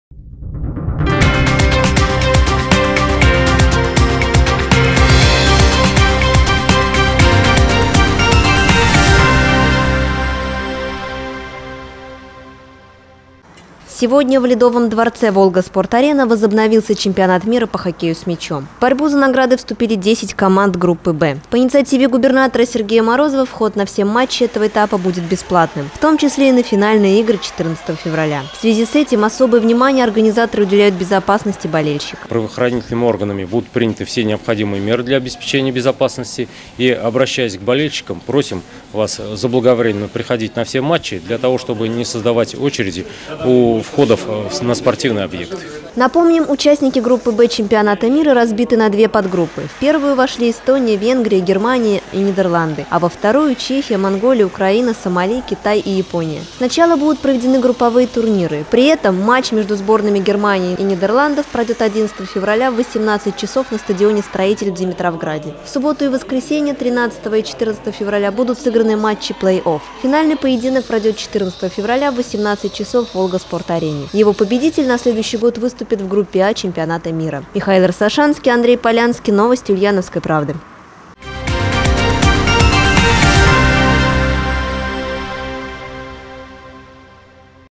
Чемпионат мира по хоккею с мячом группа «Б»
Сегодня в ледовом дворце «Волга-Спорт-Арена»возобновился чемпионат мира по хоккею с мячом. В борьбу за награды вступили десять команд группы Б. По инициативе Губернатора Сергея Морозова вход на все матчи этого этапа будет бесплатным.